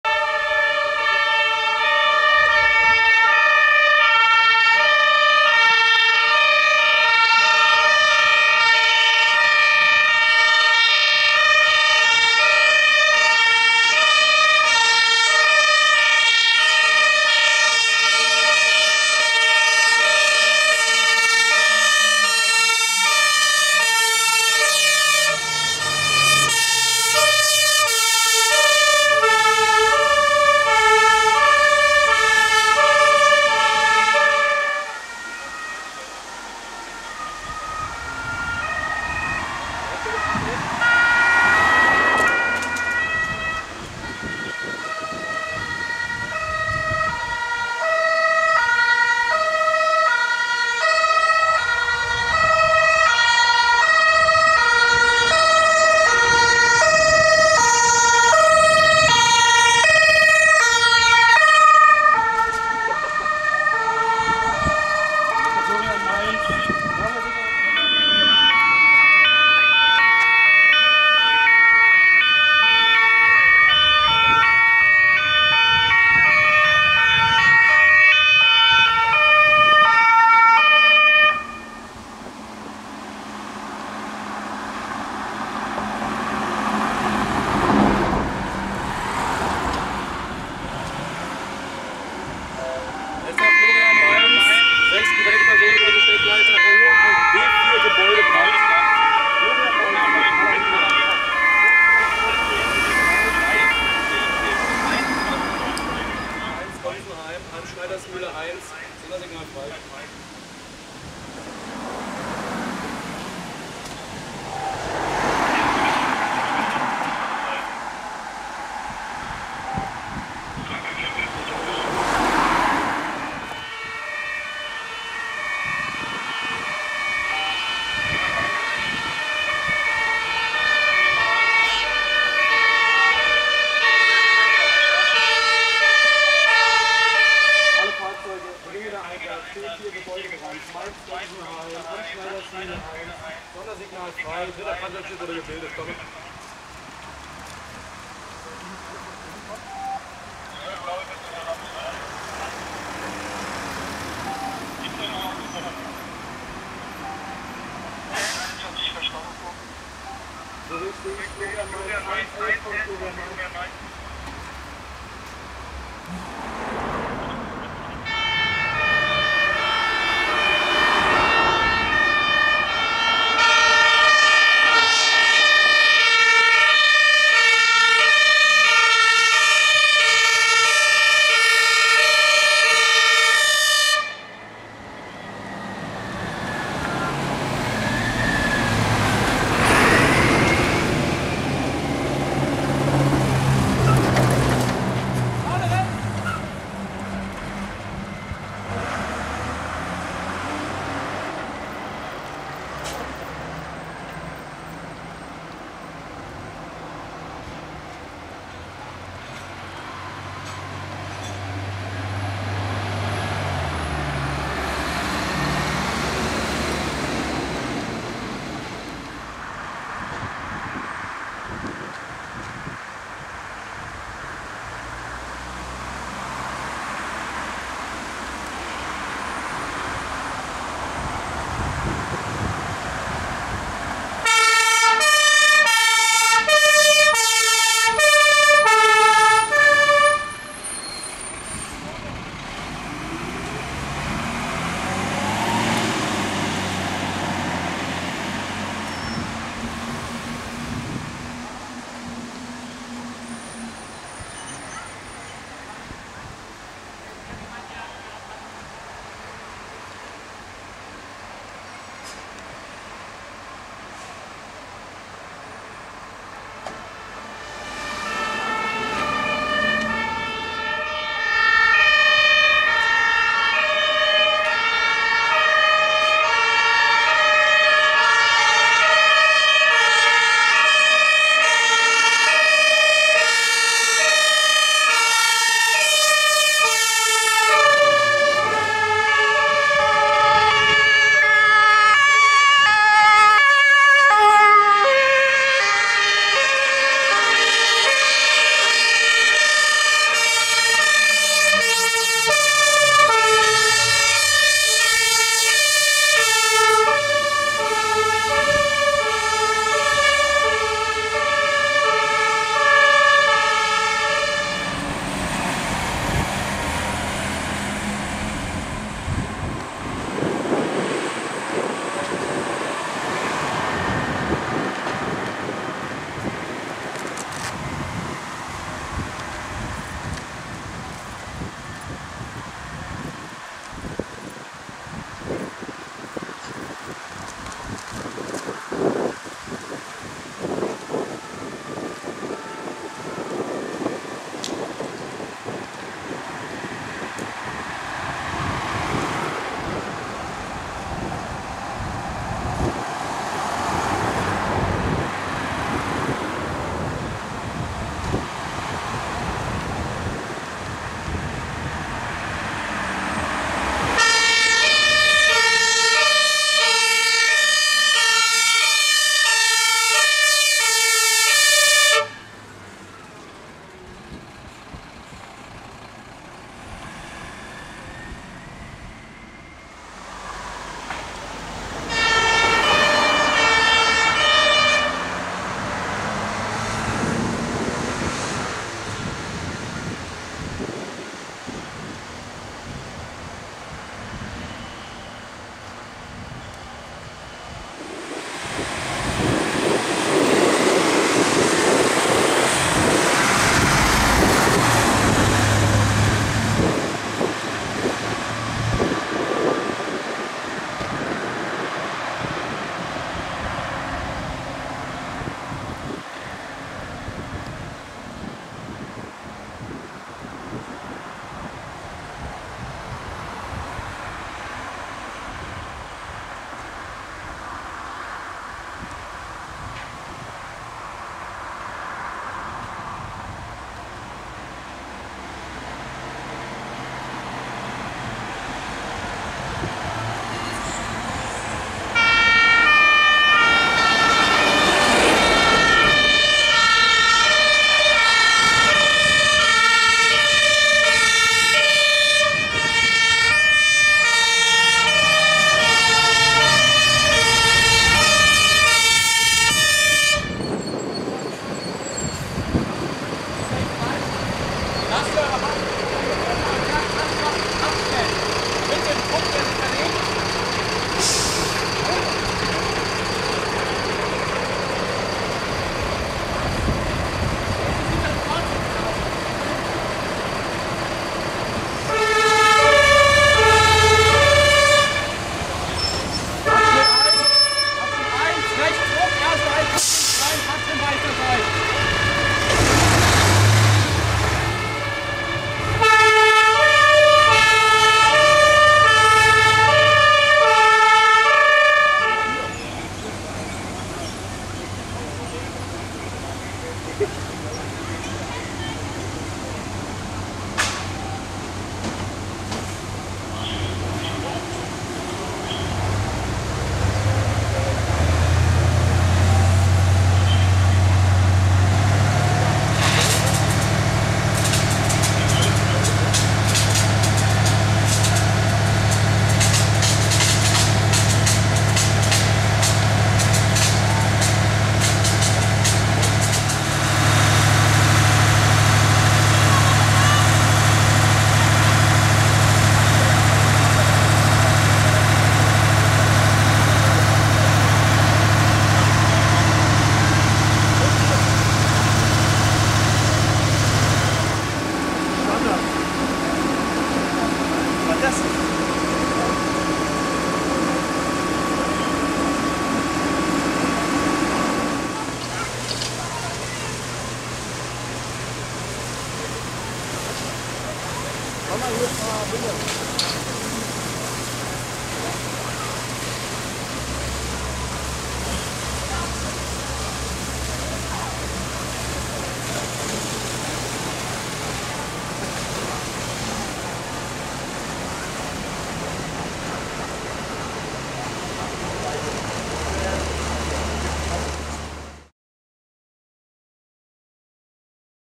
[Feuer in Schule] Abschlussübung der Mainzer Jugendfeuerwehren 2018
In diesem Video sieht man die anrückenden Einsatzkräfte zur Abschlussübung der Mainzer Jugendfeuerwehren, an einer Schule.
Gefilmt in Mainz 09.2018

Video and Copyright by Team-Firefighter5276 (MK)